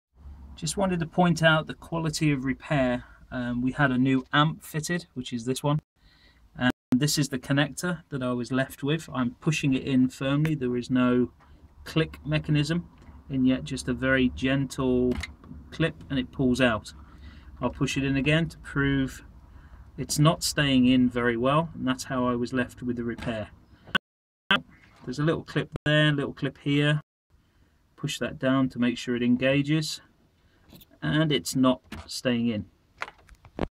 Hot tub Sound system connactor
4 Speakers had blown over last 12 months :Amp 1 was replaced with a version 2 ( with integrated gain amp) the connector was the old connector that was left in line (not replaced) and never pushed in to make a proper fit